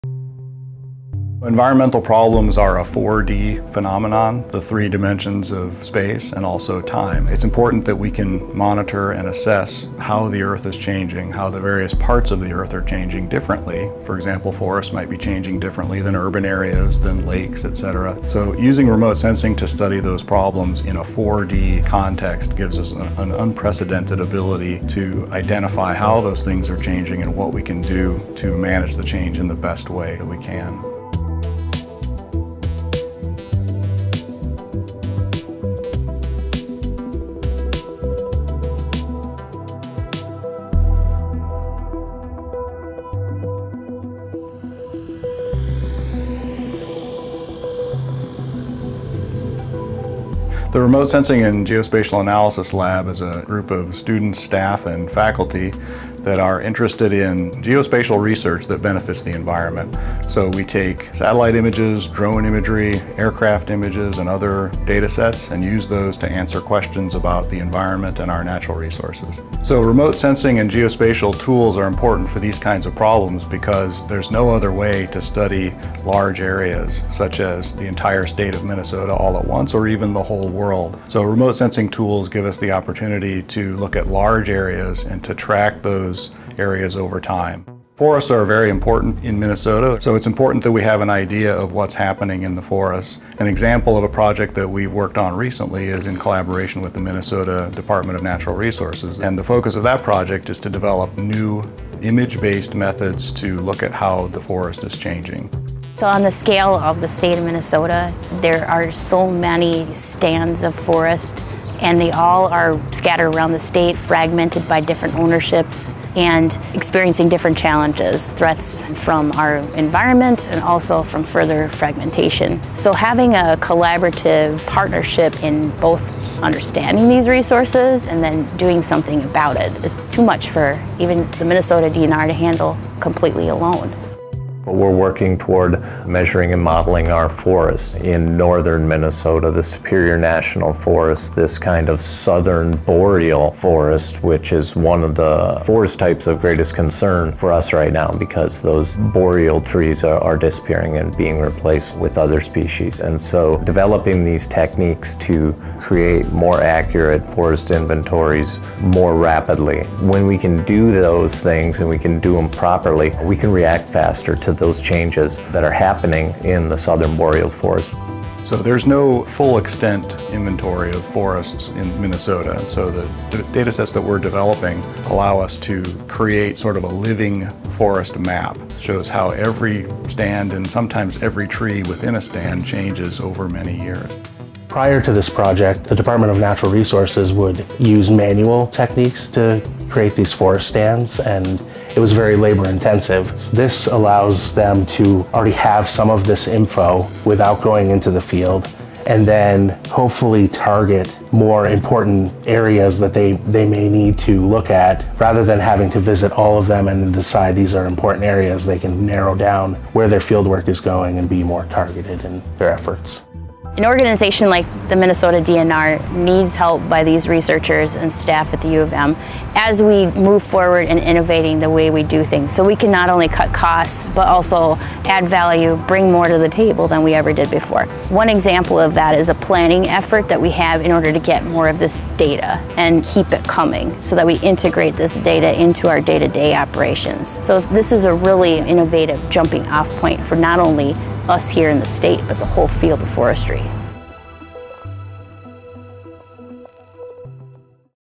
Using Remote Sensing and Geospatial Analysis to Drive Sustainable Forest Management
The Remote Sensing and Geospatial Lab at the University of Minnesota is using remote sensing and geospatial analysis in the Superior National Forest to get an unprecedented understanding of how the forest is changing over time in order to identify what can be done to manage and preserve the forest in the future.